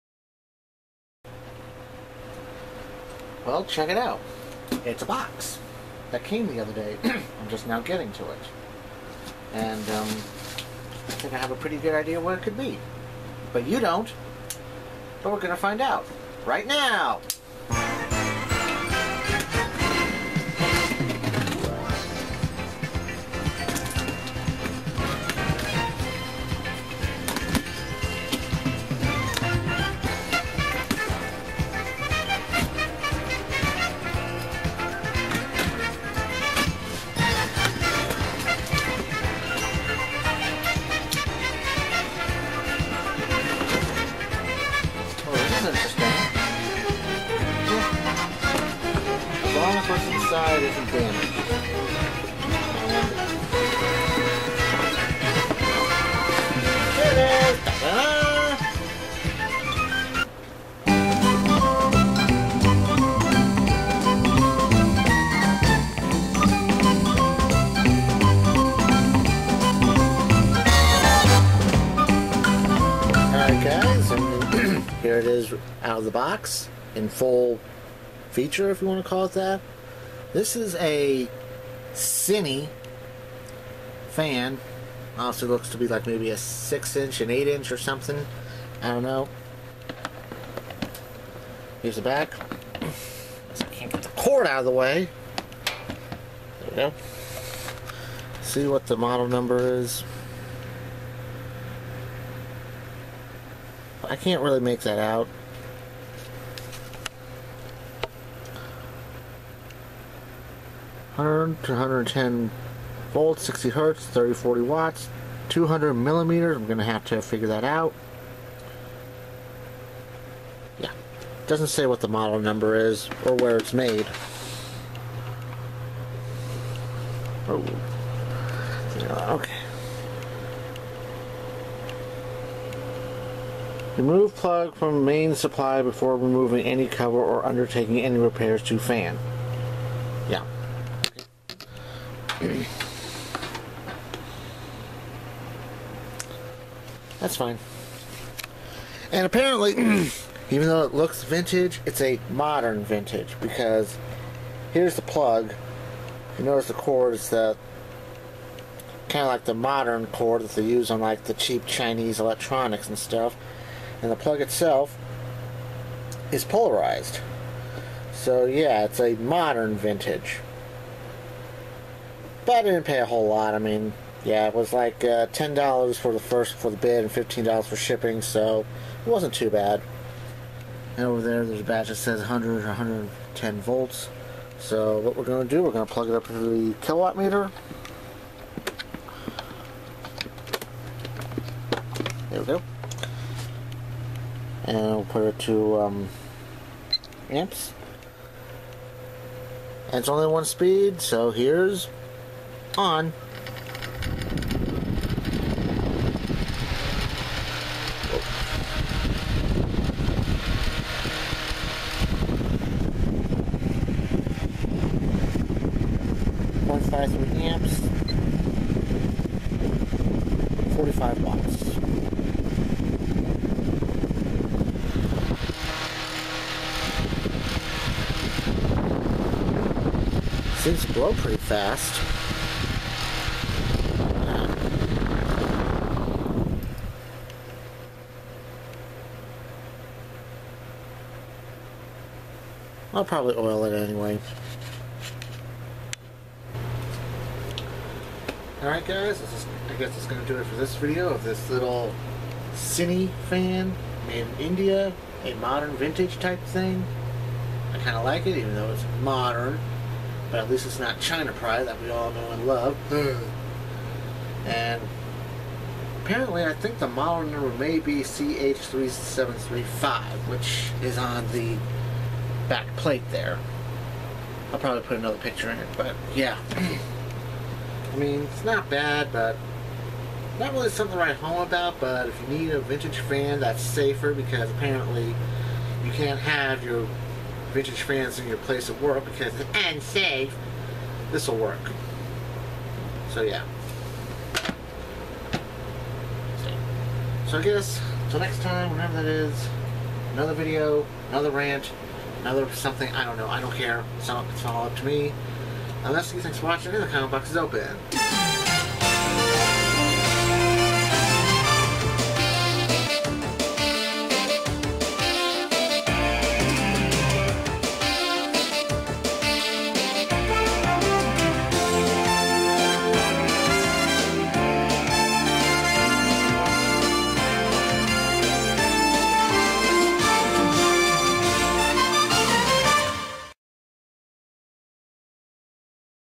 Eskie's Vlog 051515: A Modern Vintage Cinni Fan
An eBay find, as well as my first Cinni fan.